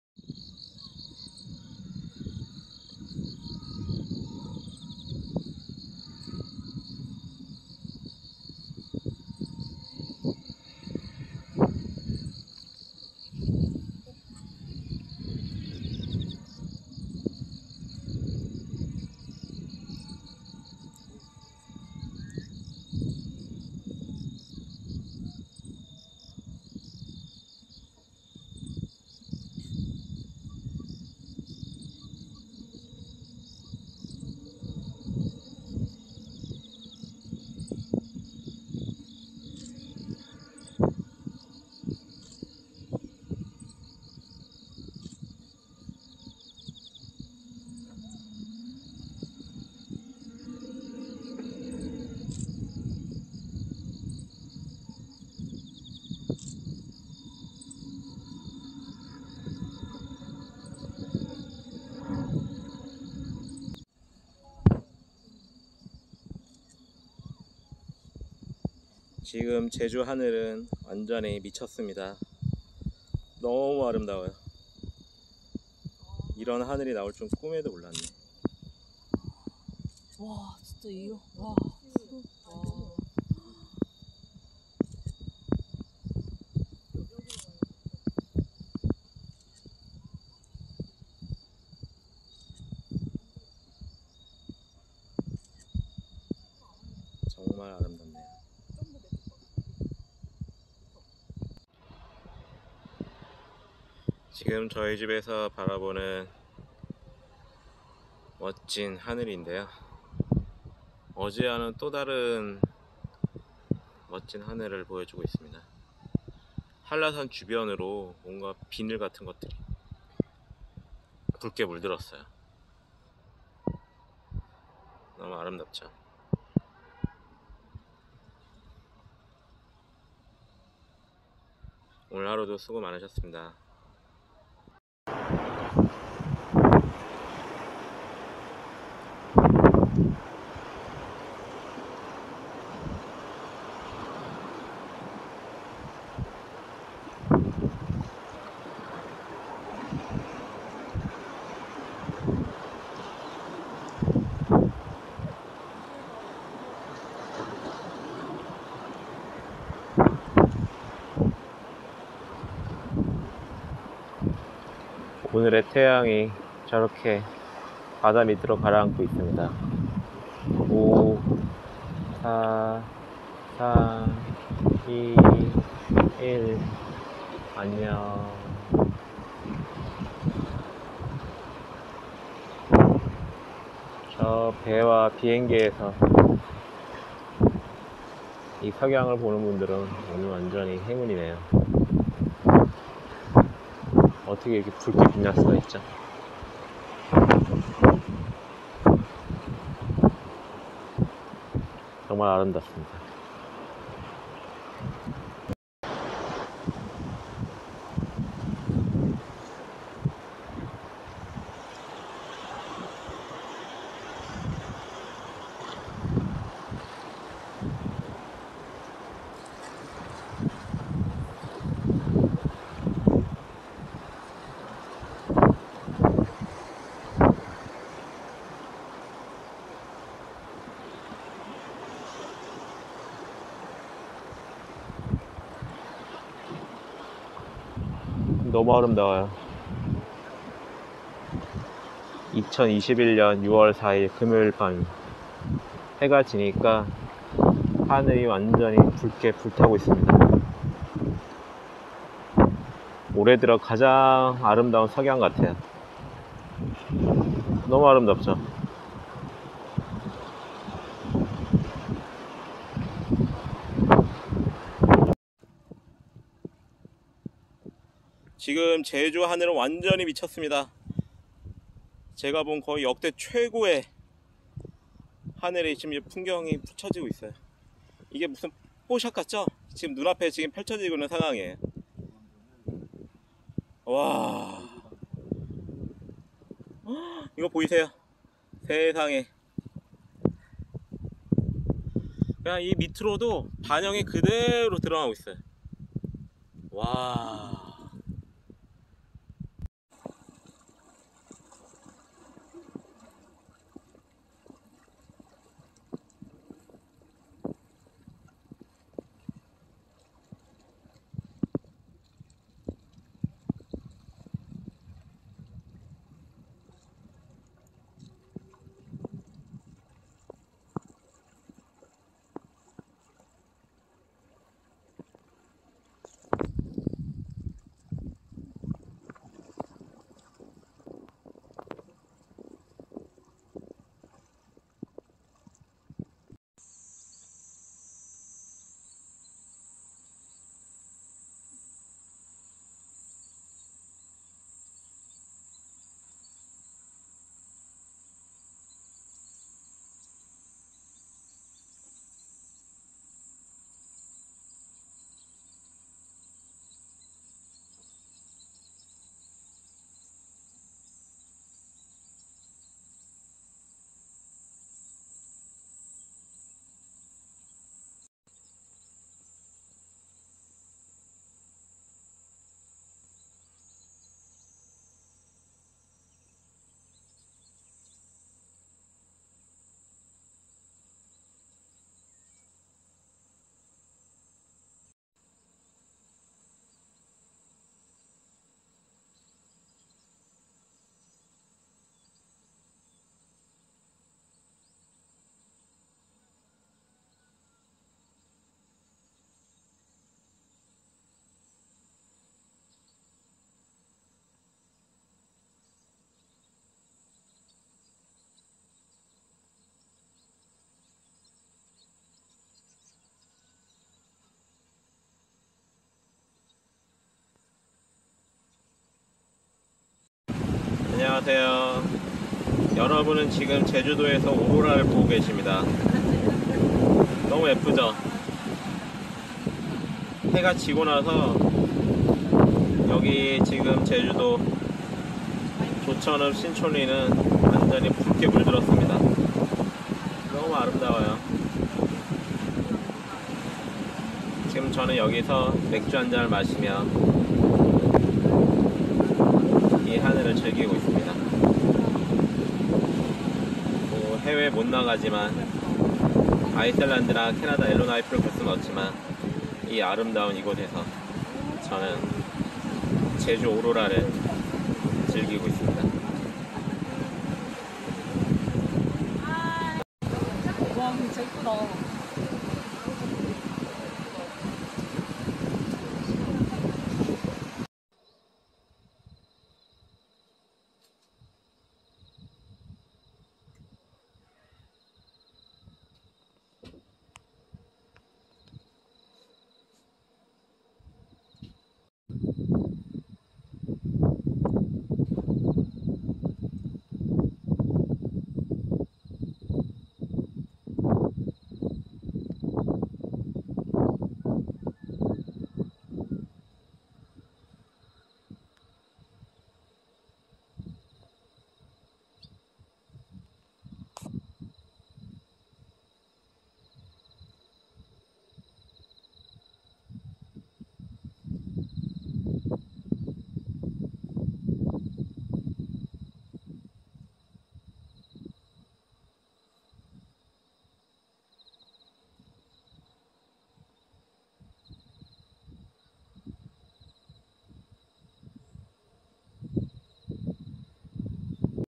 5년간 찍은 제주 일출과 일몰 모음(Sunrise and sunset of Jeju island)
5년 동안 제주에 살면서 찍은 아름다운 제주 일출과 일몰을 하나의 영상으로 모았습니다.